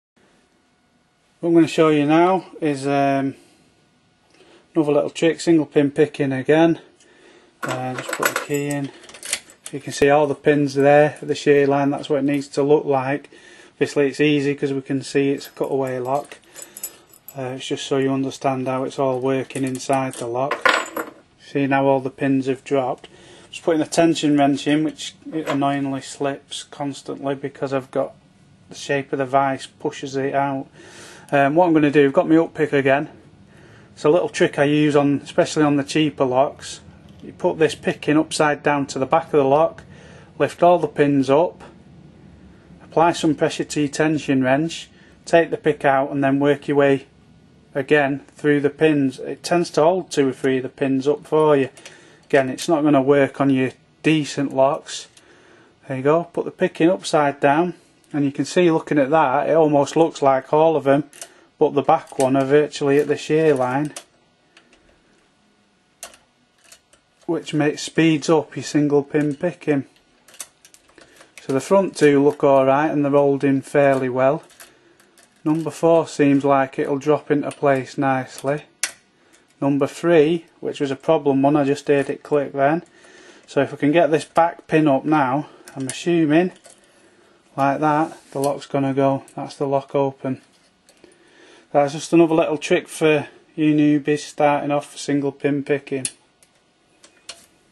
single pin pick trick for newbies.wmv
basic single pin picking for newbies to get an idea of what to do.